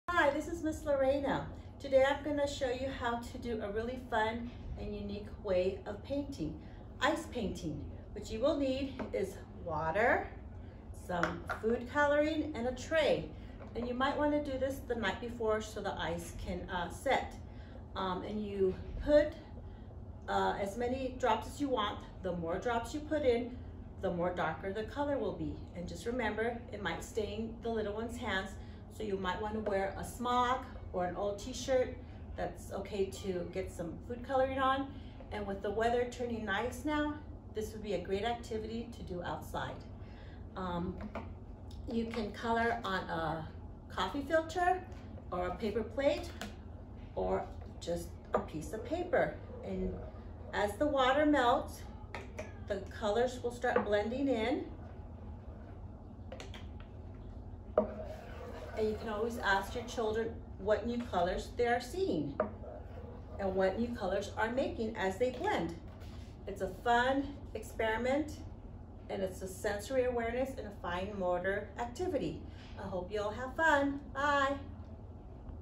Ice Cube Painting with Ms. Lorena
As the weather is heating up and the sun is shining most days, Ms. Lorena has a fun art activity to take outside using an ice cube tray, food coloring, popsicle sticks and paper.